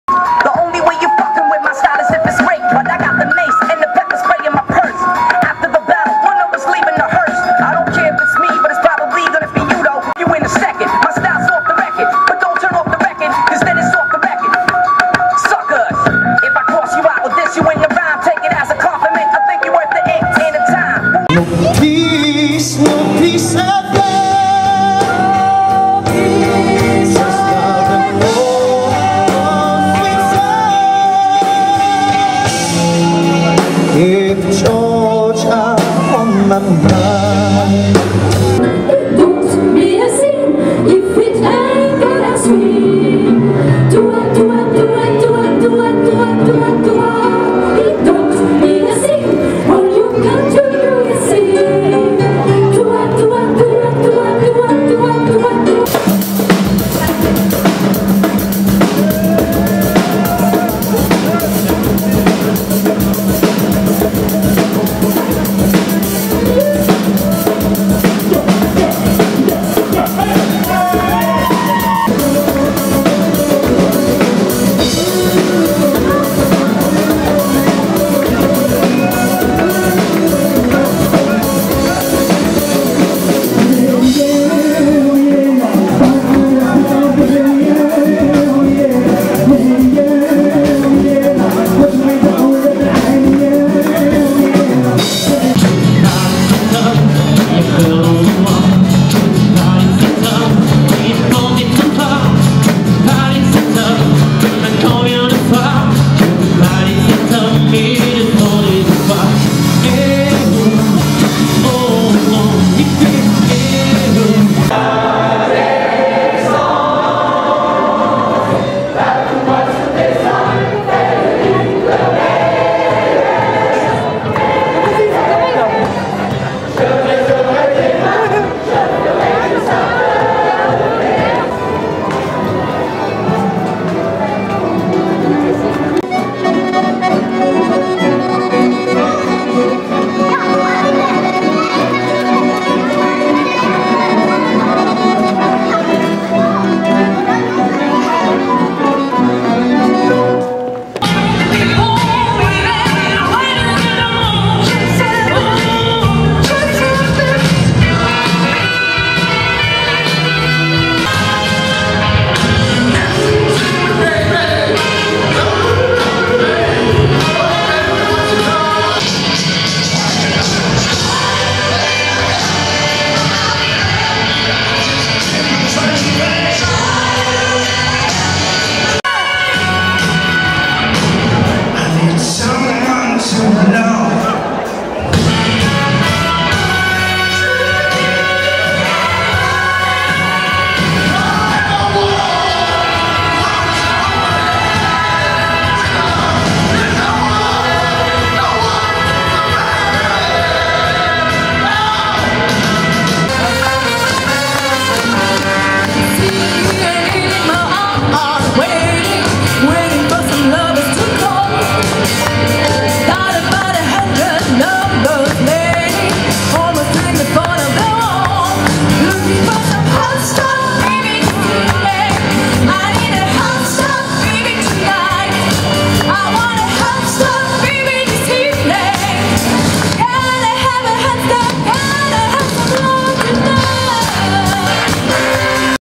Fete de la Musique 2012 Part 1
the annual France music festival on June 21st. this is 2012 in Chambery, France. Visit Chambery on our tours: